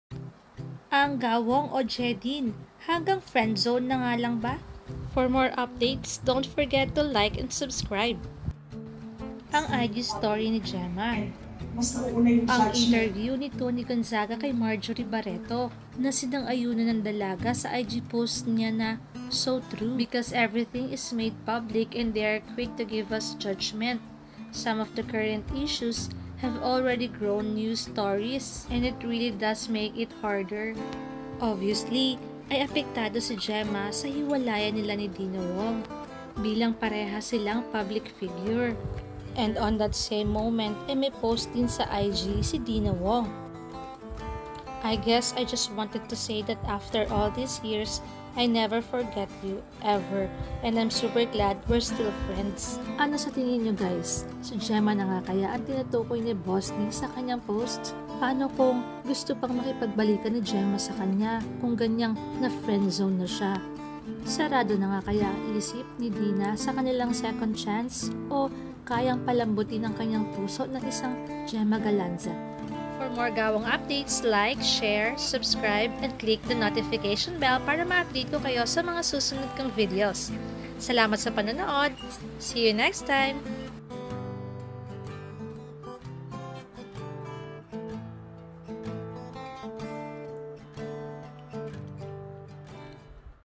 Deanna FRIEND ZONE na lang Kay JEMA🤔 Kaya ba may HUGOT si GALANZA sa IG STORY 😢WATCH THIS‼️
GAWONG PALITAN NG HUGOT sa IG Story Sept.8, 2021...aNG TOTOO ALAMIN!